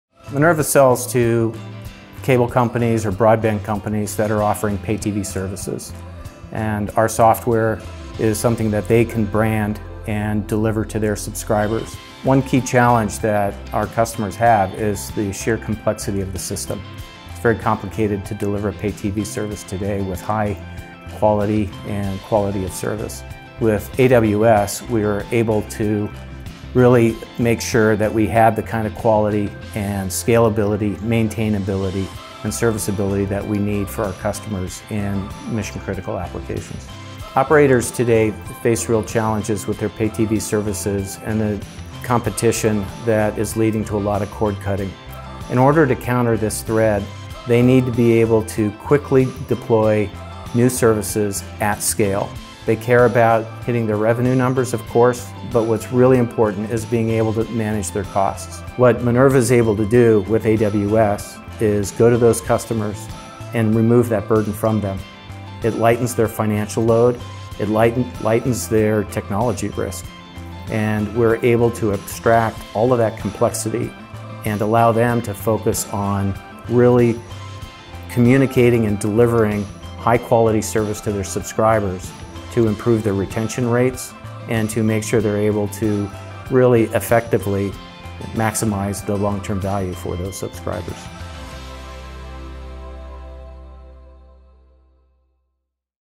How Minerva Networks Provides Customer Value with AWS
Minerva Networks provides service management solutions for the delivery of advanced pay television services. "With AWS, we are able to make sure that we have the kind of quality, scalability, maintainability and serviceability that we need for our customers and mission critical applications," says Matt Cuson, VP of Product and Marketing at Minerva Networks. "What Minerva is able to do with AWS is lighten our customer's financial load and lighten their technology risk."